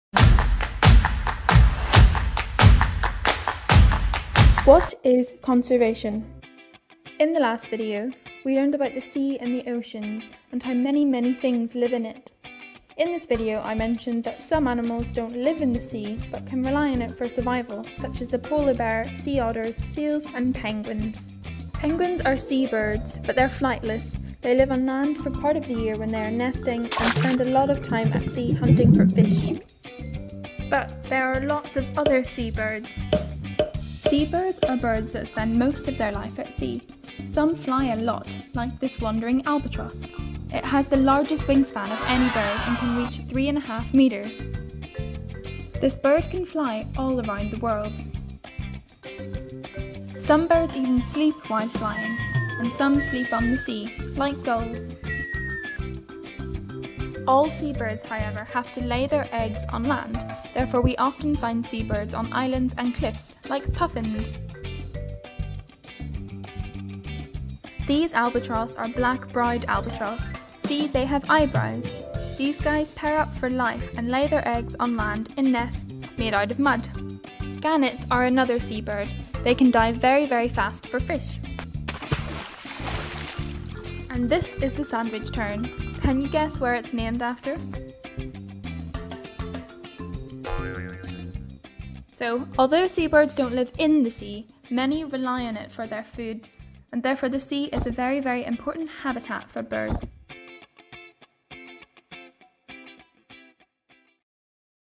What is Conservation? Episode 4 - Sea Birds
Welcome back to our video series "What is Conservation?" 🔍🌳This episode focuses on sea birds, and describes a few birds that the seas and oceans are important habitats for! 🐧🌊🐦

I'm loving making them and learning while I'm doing so. ✏️🍃